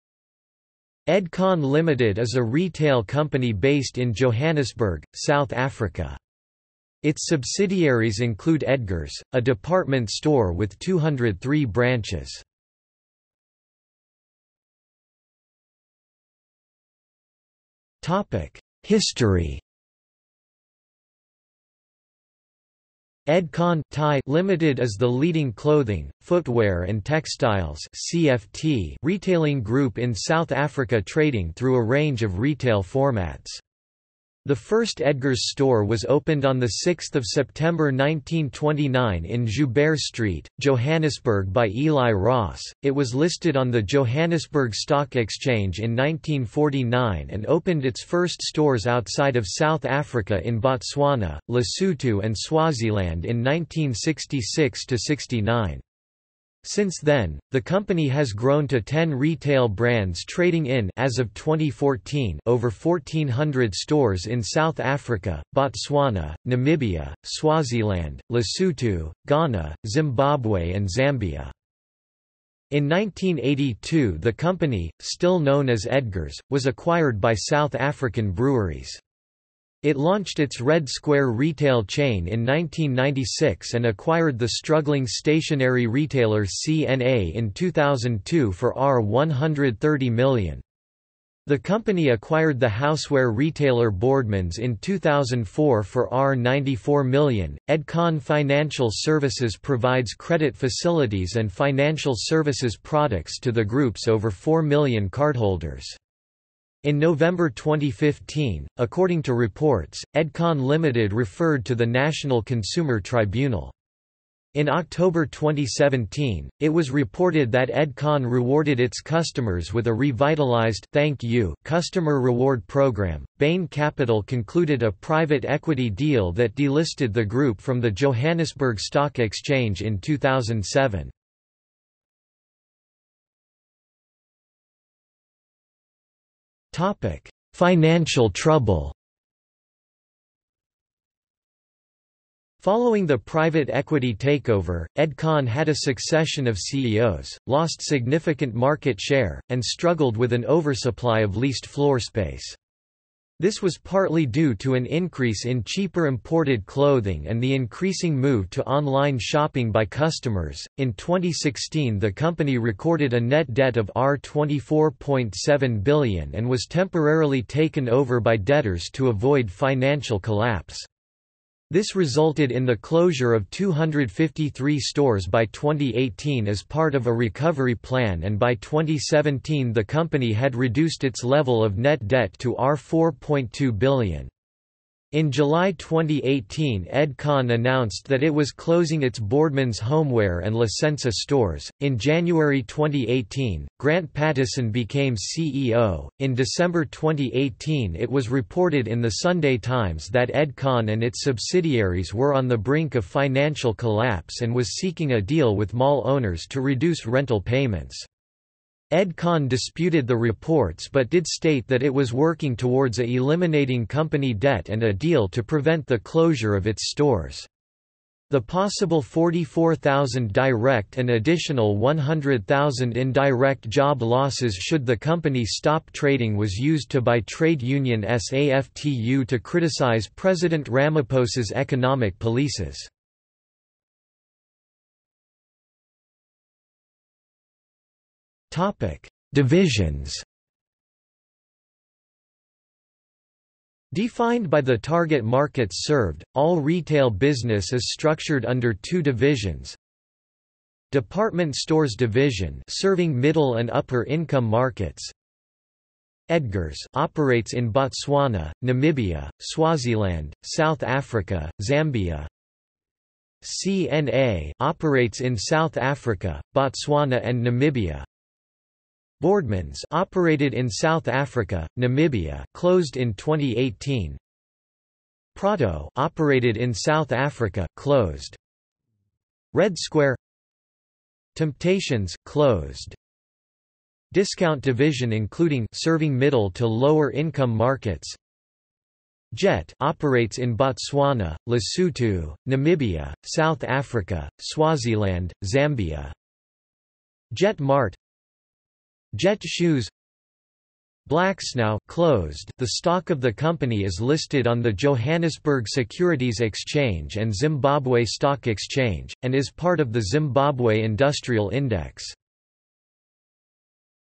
Edcon Limited | Wikipedia audio article
00:00:14 1 History
00:02:13 2 Financial trouble
00:03:54 3 Divisions

- Socrates

SUMMARY
Edcon Limited is a retail company based in Johannesburg, South Africa.  Its subsidiaries include Edgars, a department store with 203 branches.